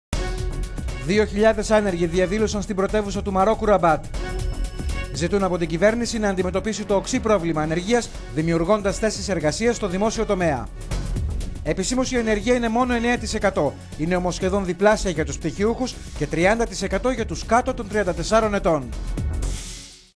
Μαρόκο: Διαδήλωση ανέργων
Δύο χιλιάδες άνεργοι διαδήλωσαν στην πρωτεύουσα του...

Δύο χιλιάδες άνεργοι διαδήλωσαν στην πρωτεύουσα του Μαρόκου, Ραμπάτ.

Ζητούν από την κυβέρνηση να αντιμετωπίσει το οξύ πρόβλημα ανεργίας, δημιουργώντας θέσεις εργασίας στο δημόσιο τομέα.

Επισήμως η ανεργία είναι μόνο 9%.